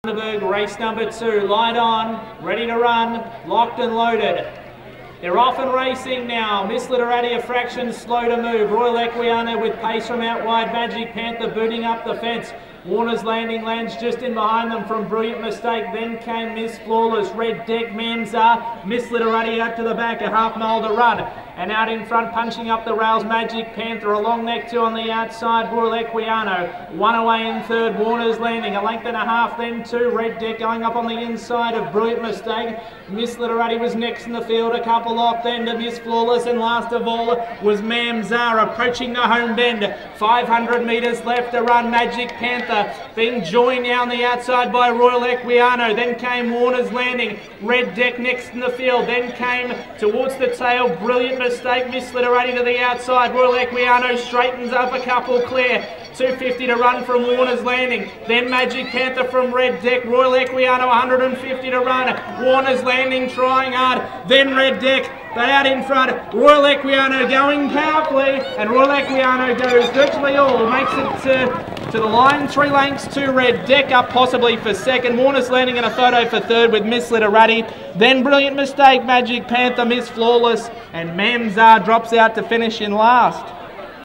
Race 2: Formatt Machinery QTIS Class 2 Plate 1090m.
Winner: ROYAL EQUIANO
Trainer: Cherie Vick
Jockey: Robert Faehr

2nd: Red Deck
3rd: Miss Literati

Results: 7, 5, 6, 1.